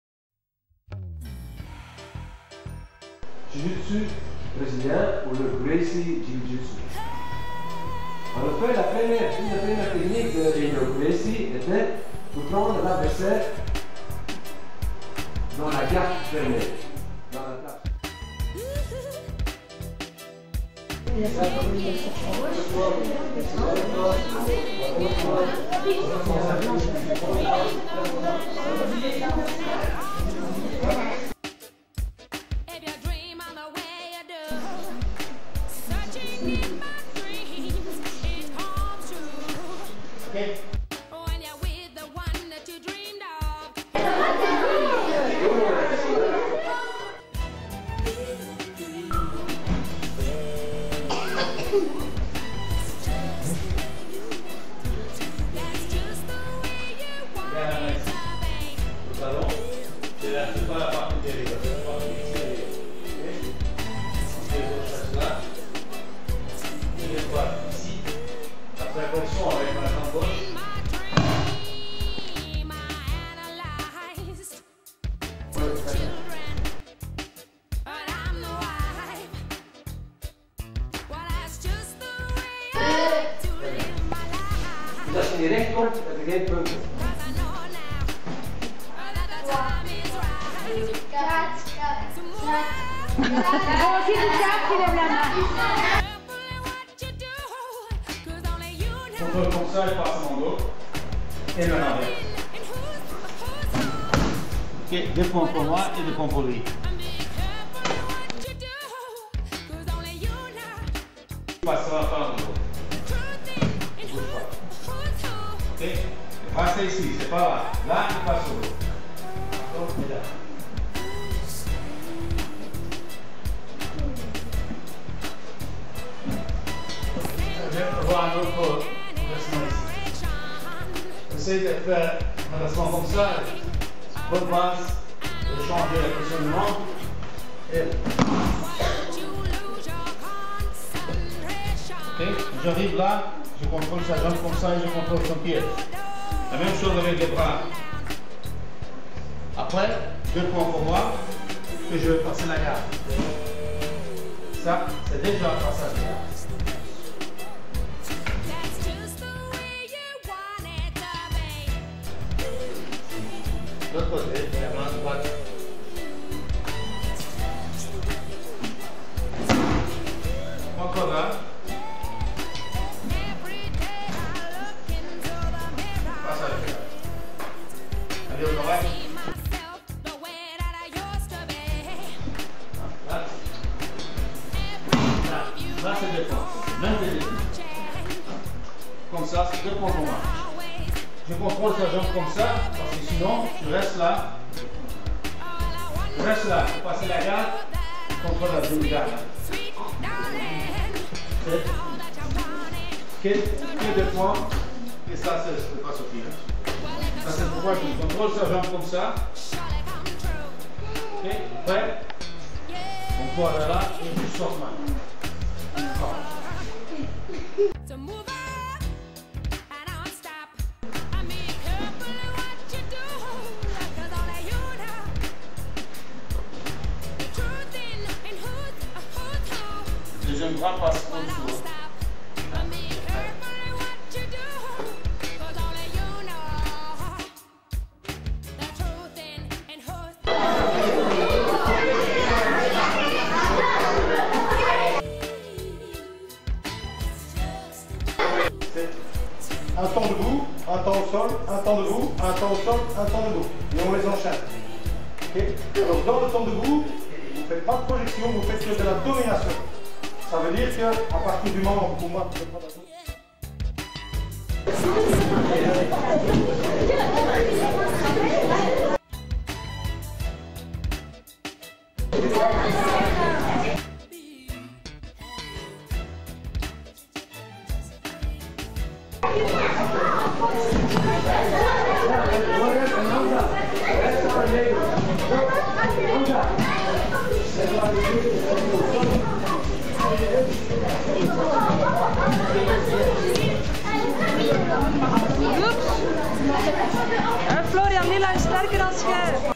flavio behring (9dan BJJ) teaching kids in Brussels some Sweeps
Flavio Behring visiting the Brussels Brazilian Jiu Jitsu Academy, 11 november 2009.